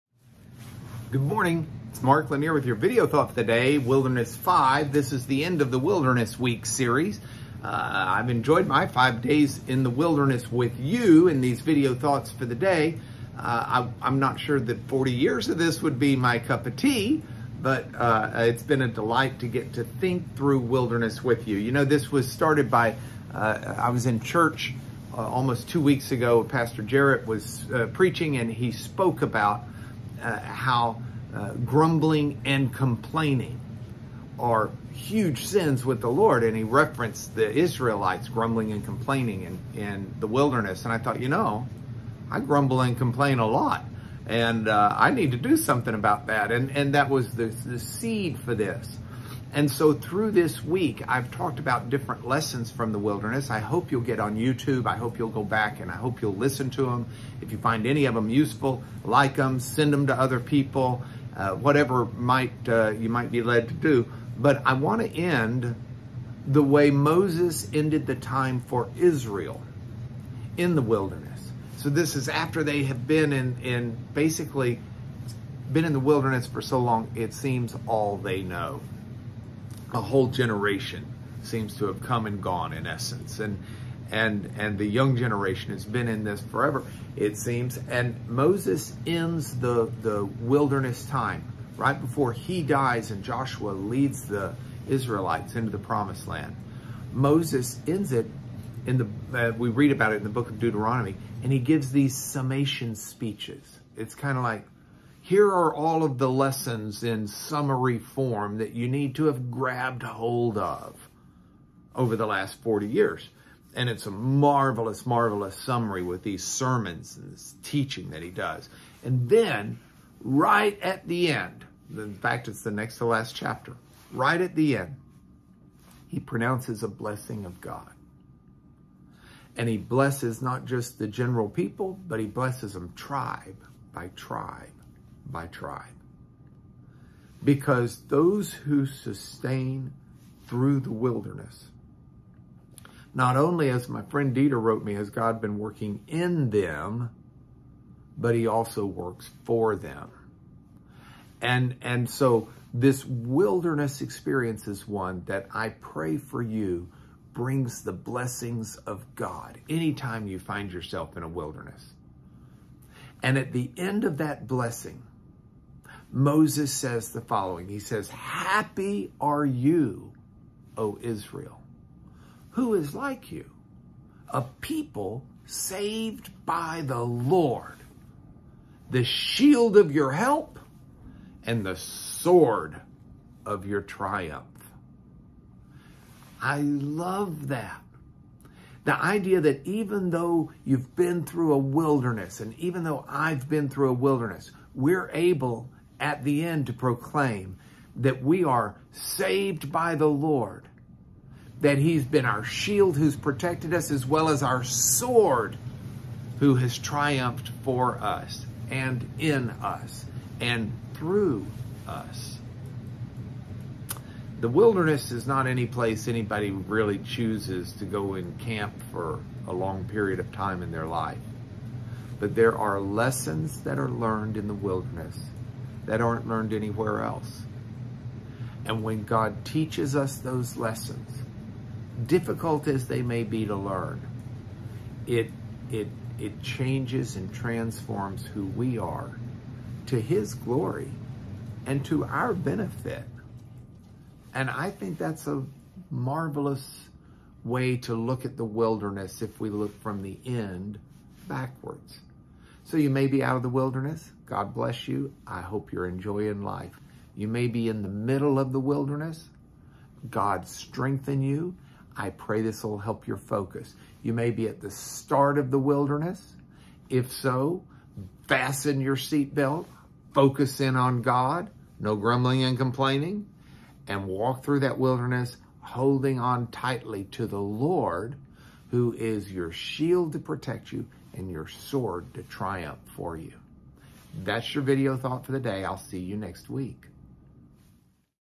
Mark's Thought For The Day - 10/22/21 “Wilderness 5: The Good Part"
Did you know that grumbling and complaining are one of the worst things you can do in the eyes of the Lord?

Yeah.

It's a damaging activity and can become a habit.

Continuing the story of the Israelites in the wilderness, Mark gives the truth of peace and triumph because of the lessons God can teach us only during that time. And learn not to grumble as much as you can.

Watch and Listen!